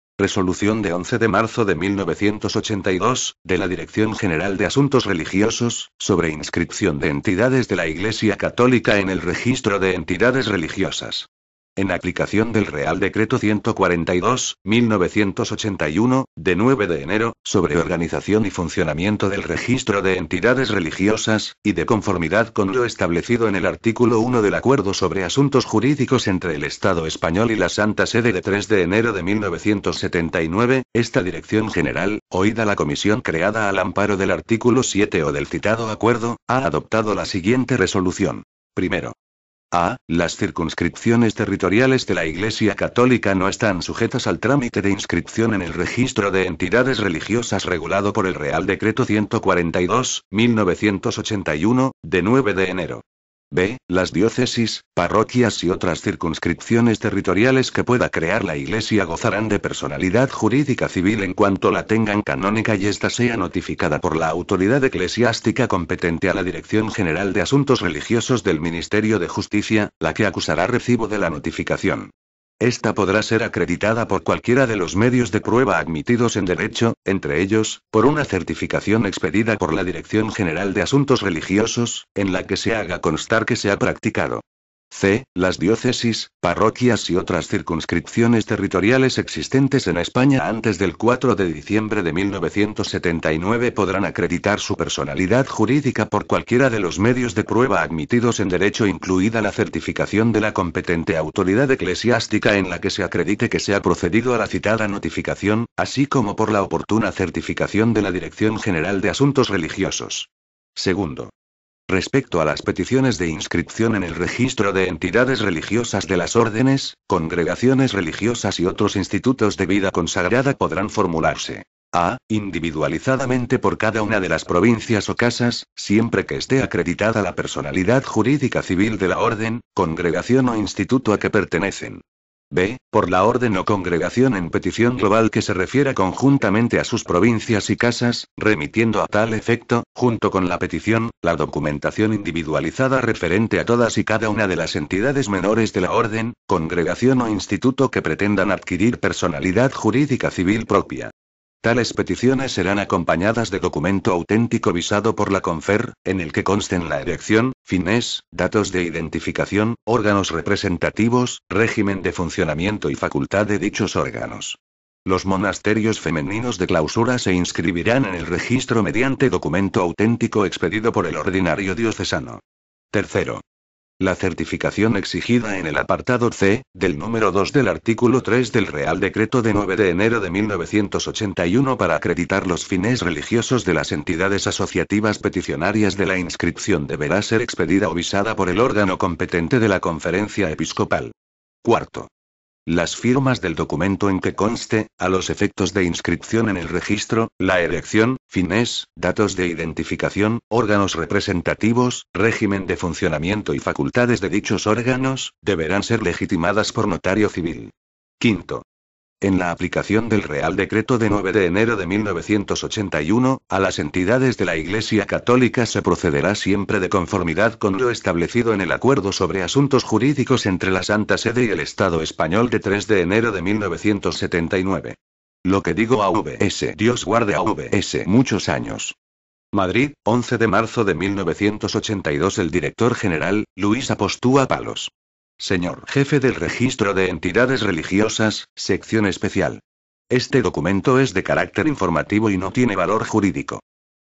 Inscripción de Entidades de la Iglesia católica en el Registro de Entidades Religiosas.
Resolución de 11 de marzo de 1982, de la Dirección General de Asuntos Religiosos, sobre inscripción de Entidades de la Iglesia católica en el Registro de Entidades Religiosas.